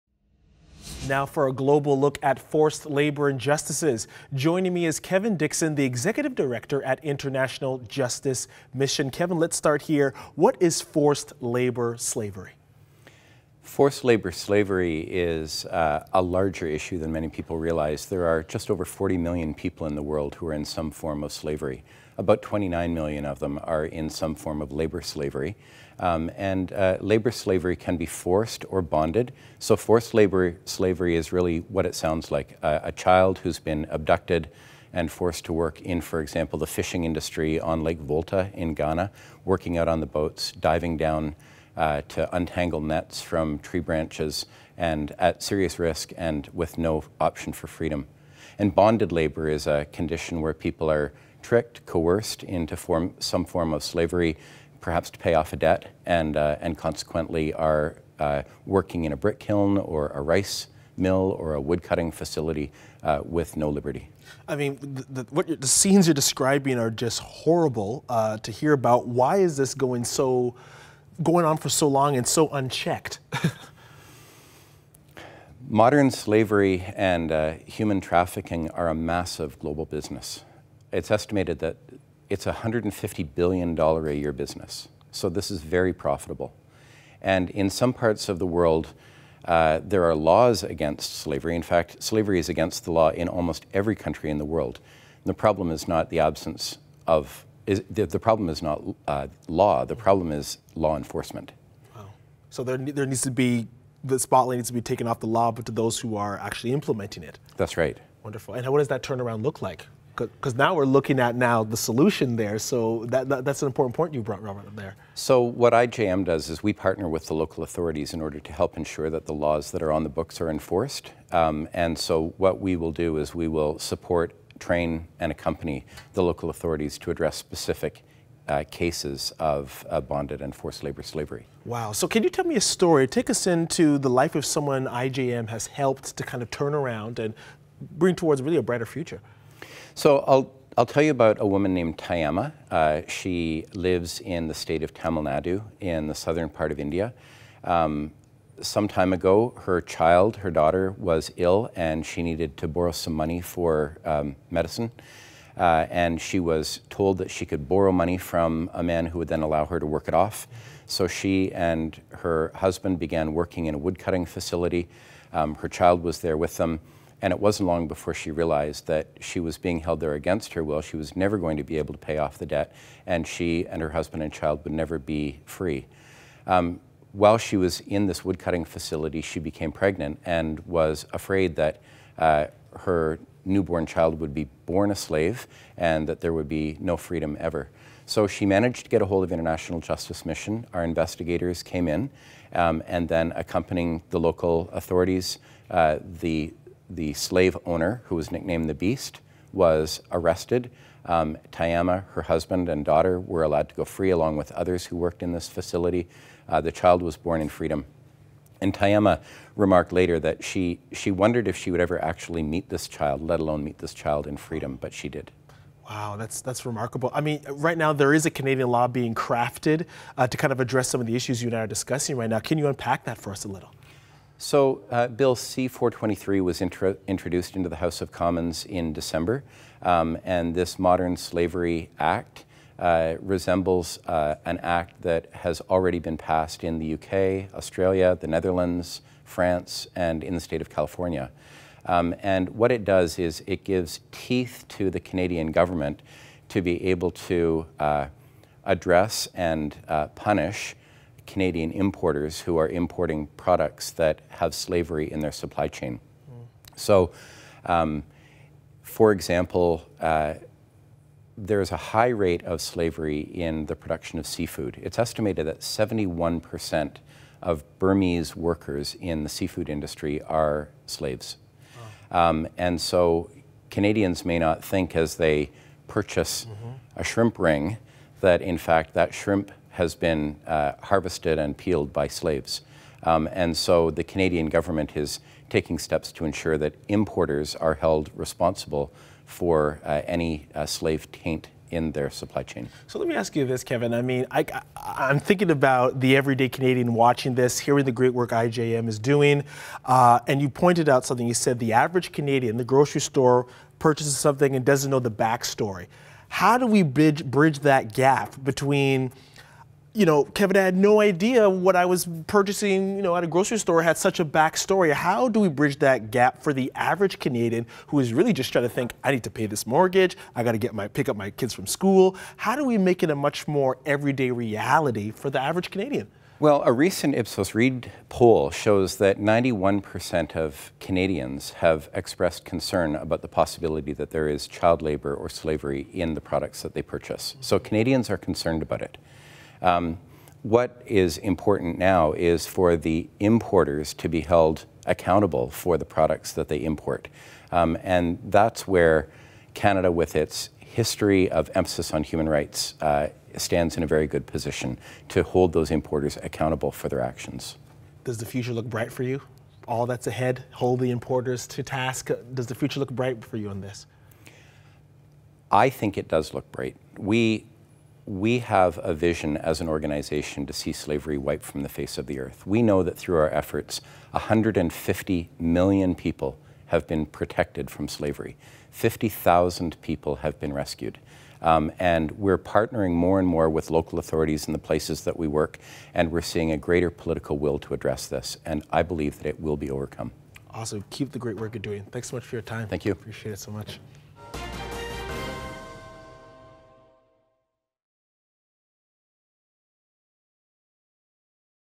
The scourge of forced and exploited migrant workers
Context Beyond the Headlines speaks with the executive director of International Justice Mission, Kevin Dixon who gives a global look at forced labour injustices happening around the world.